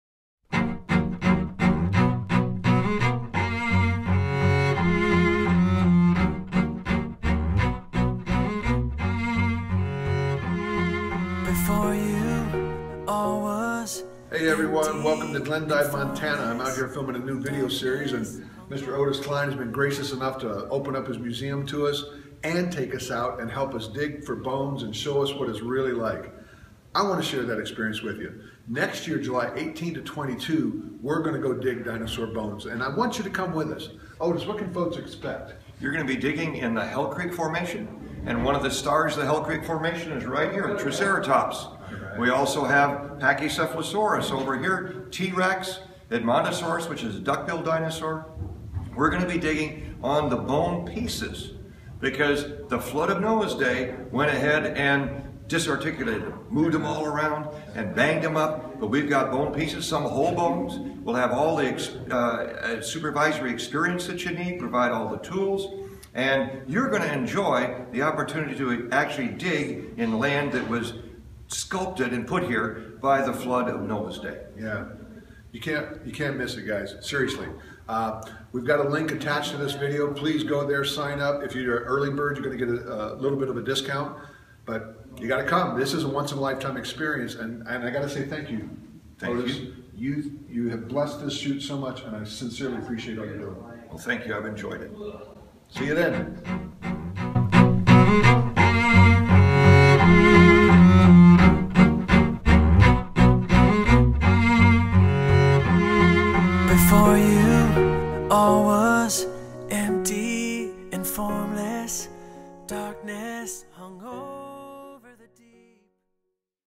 Dig Advertisement
Join Carl Kerby and Reasons for Hope for a Dinosaur Bone Dig in Glendive Montana.  July 18-22, 2016